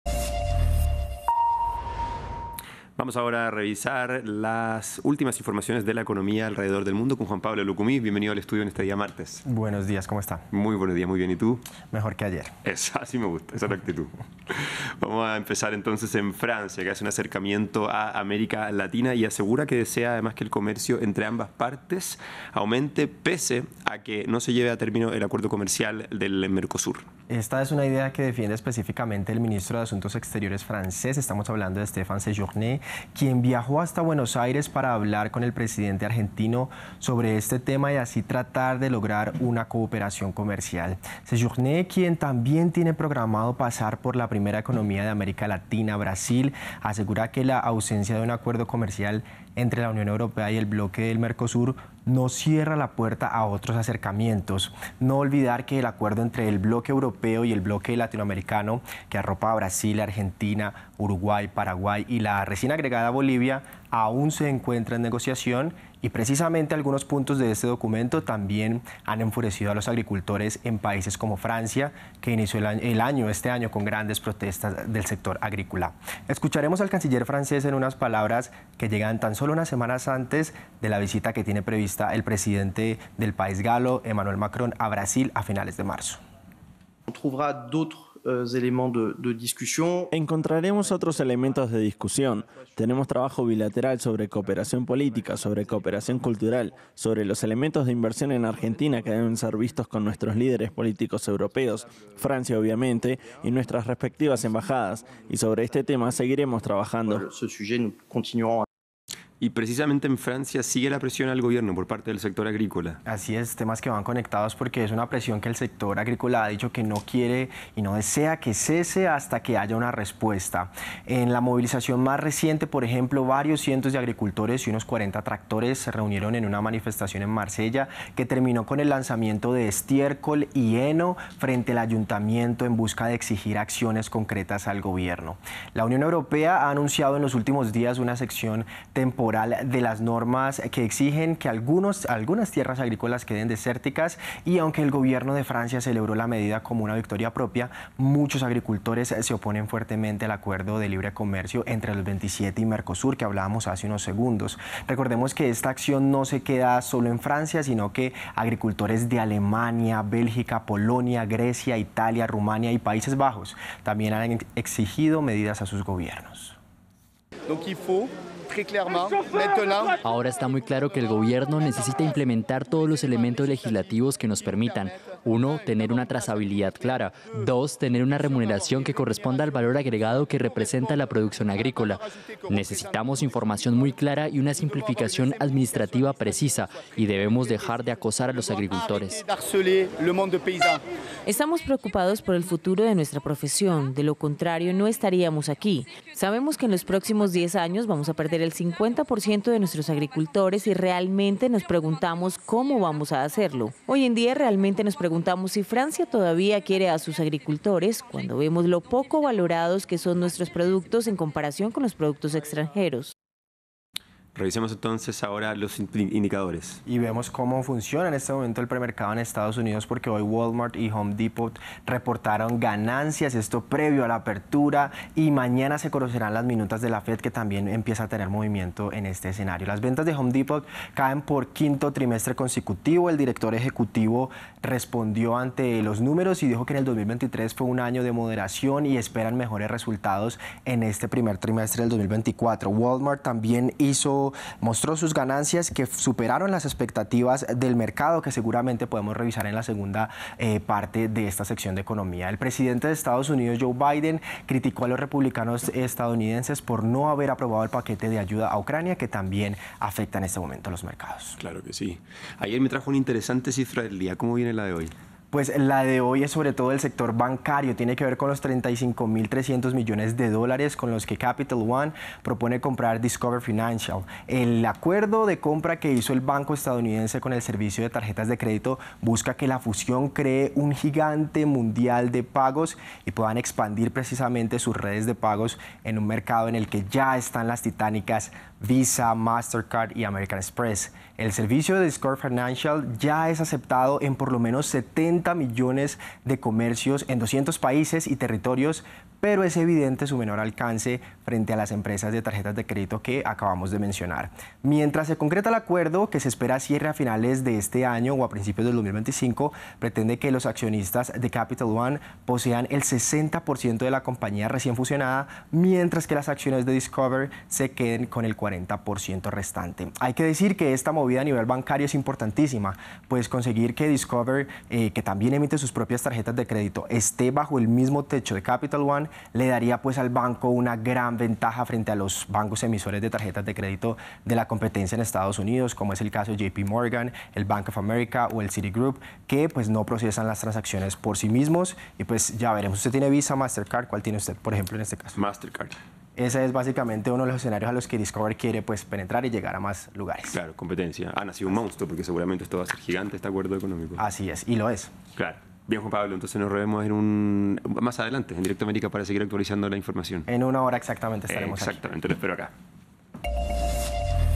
Francia se opone al acuerdo con Mercosur pero no descarta negociaciones alternas con América Latina
En un acercamiento a las principales economías de la región, el canciller francés visitó Argentina y espera llegar a la potencia Latinoamericana, Brasil. El Gobierno del presidente Emmanuel Macron ha dicho que, independientemente de lo que suceda con el acuerdo entre la Unión Europea (UE) y Mercosur, al que se opone firmemente, no hay que descartar el potencial comercial que tiene América Latina y que se encontrarán los métodos para lograr negociaciones en el futuro.